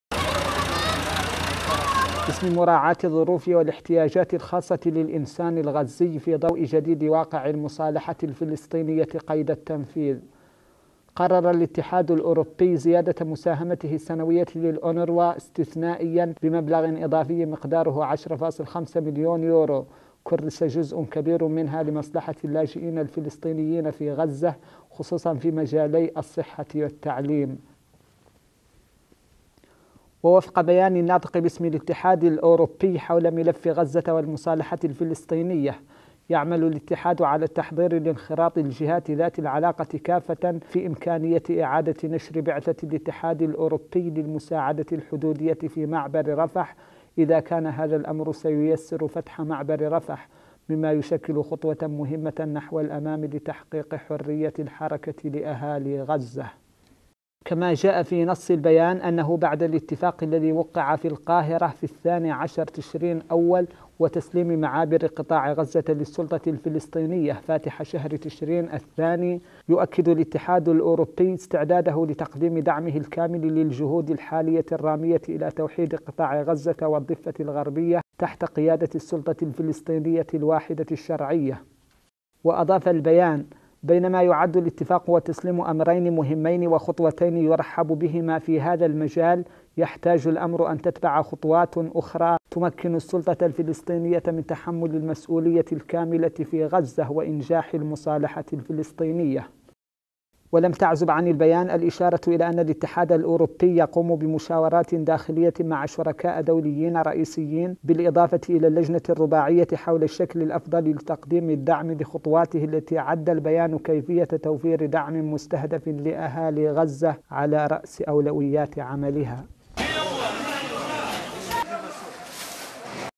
الاتحاد الأوروبي يقرر زيادة مساهمته السنوية للأونوروا تقرير اشريف ولد محمد يحيى.
قرر الاتحاد الأوروبي زيادة مساهمته السنوية للأونروا بمبلغ إضافي بقيمة عشرة ملايين وخمس مائة ألف يورو، خصص جزء منها لمصلحة اللاجئين الفلسطينيين بغزة، خصوصا في مجالي الصحة والتعليم.
وقال بيان صادر عن الناطق باسم الاتحاد الأوروبي حول ملف غزة والمصالحة الفلسطينية إن الاتفاق الموقع في القاهرة يؤكد استعداده لتقديم دعمه الكامل للجهود الرامية إلى توحيد غزة.
تقرير: اشريف ولد محمد يحيى.